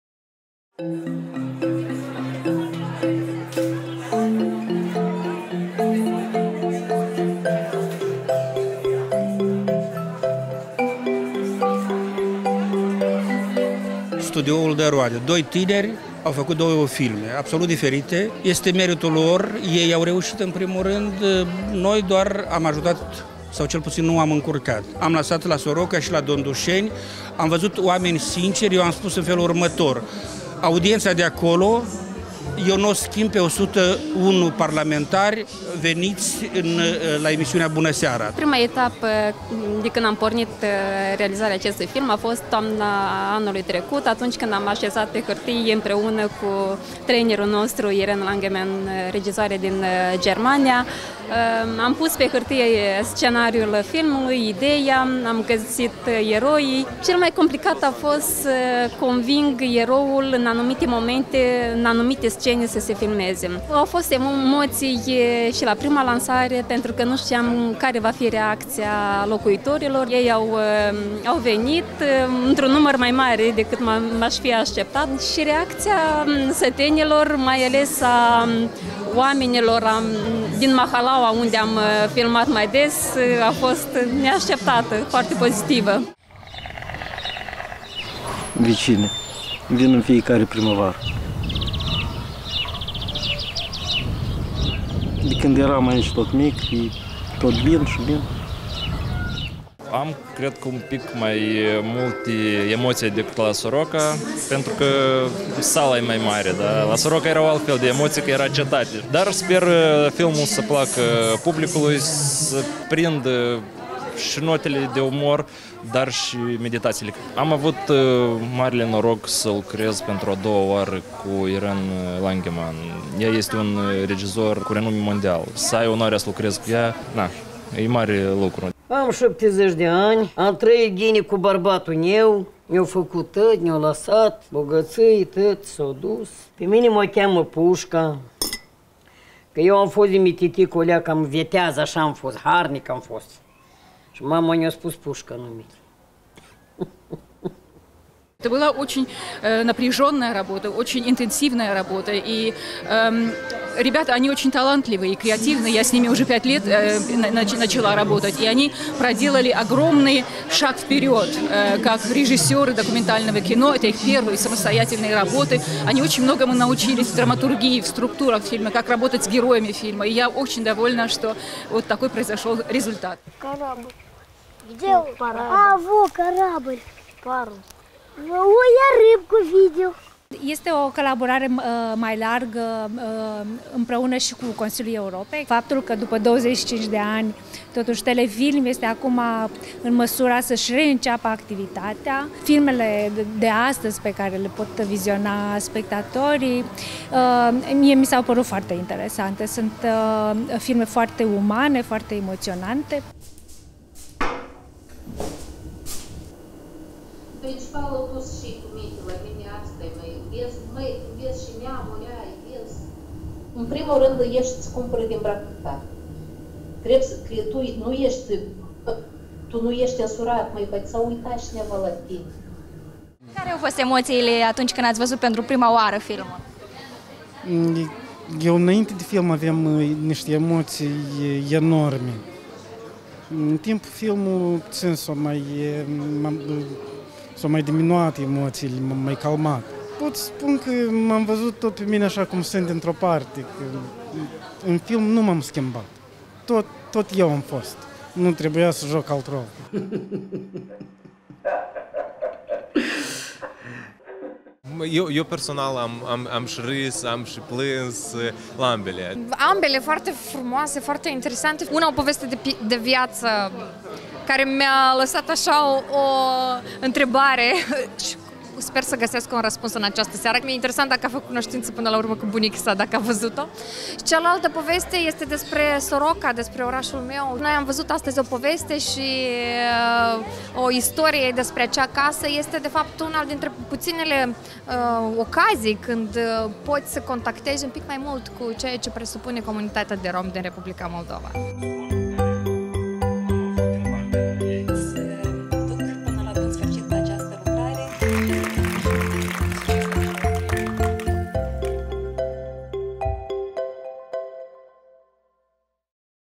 Studioul Telefilm Chişinău a lansat două filme documentare
Cele două producţii „Legământ”, semnată de Lucia Tăut, și „Teatrul Mare din Soroca” - de Eugeniu Popovici au fost realizate împreună cu regizoarea Irene Langemann, iar proiectul a fost susținut de Consiliul Europei.